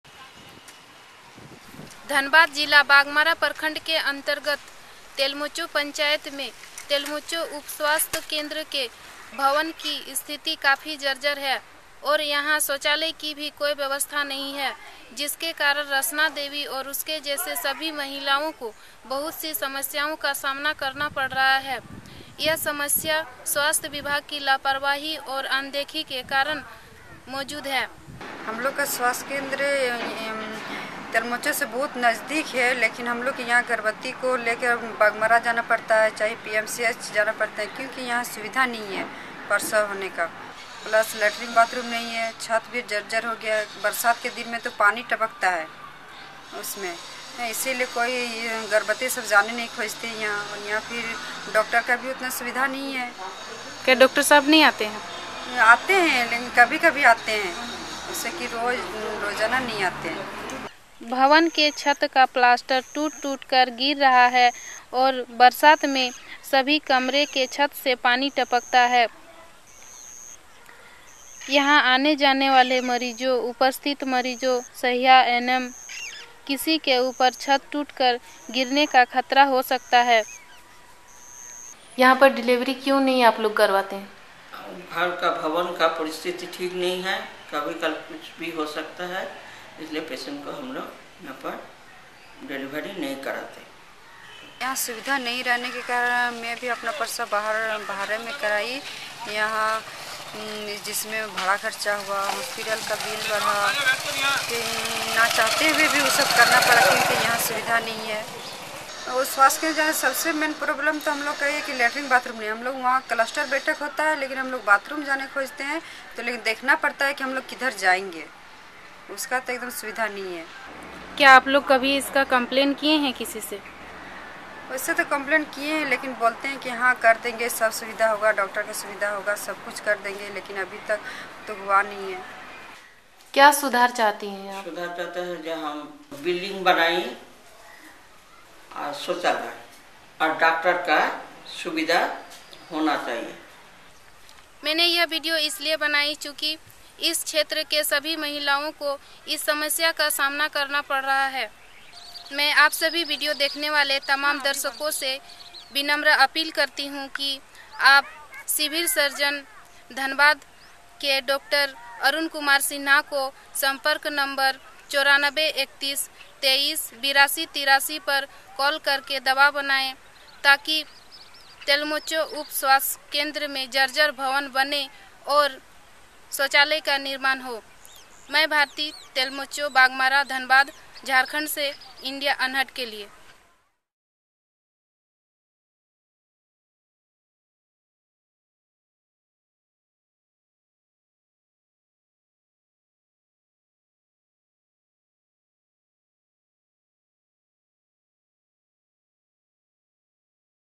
You can’t Deliver Your Baby Here, The Roof May Collapse Any Moment
Crumbling infrastructure at a state-run health facility keeps pregnant women from availing healthcare services here. With a roof that leaks during the monsoon and no functional toilets, both frontline health workers and patients find it difficult to use. Bharti Kumari Reports from Telmocho village, Dhanbad district, Jharkhand
This series on community monitoring of maternal health in India is supported by Oxfam India.